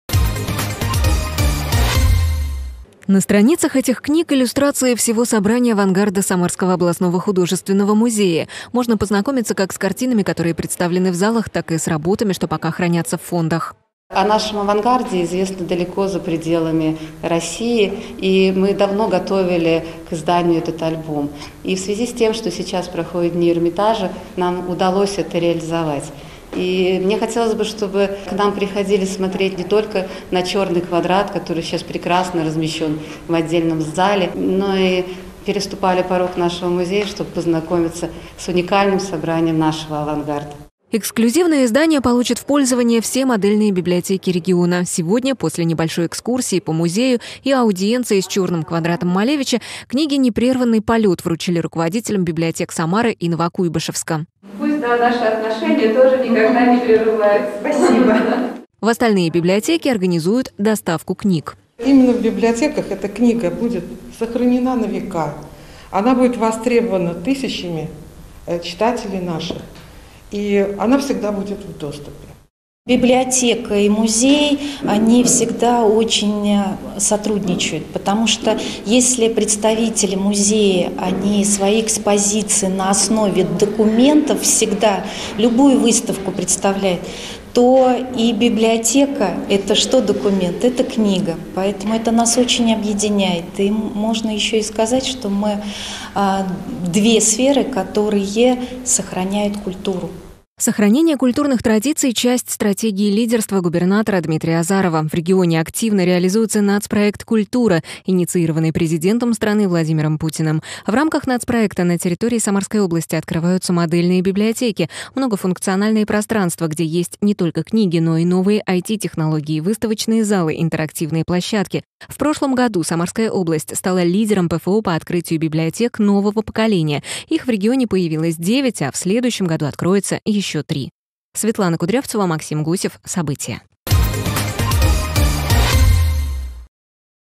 Все модельные библиотеки получат книги с иллюстрациями коллекций Самарского художественного музея
Вышли в авангард! Все модельные библиотеки региона получат книги с иллюстрациями коллекций Самарского художественного музея. Эксклюзивные  издания сегодня вручили руководителям библиотек Самары и Новокуйбышевска. Дату для этого выбрали не случайно. 11 декабря 1871 года в Петербургской Академии художеств открылась первая выставка передвижников,  работы которых сейчас можно увидеть в самарском музее,  а ровно 105 лет назад, в декабре 1915-го  широкой публике впервые представили самую знаменитую картину русского авангарда - "Чёрный квадрат" Казимира Малевича. И сейчас один из квадратов тоже можно увидеть в художественном музее. Светлана Кудрявцева продолжит тему.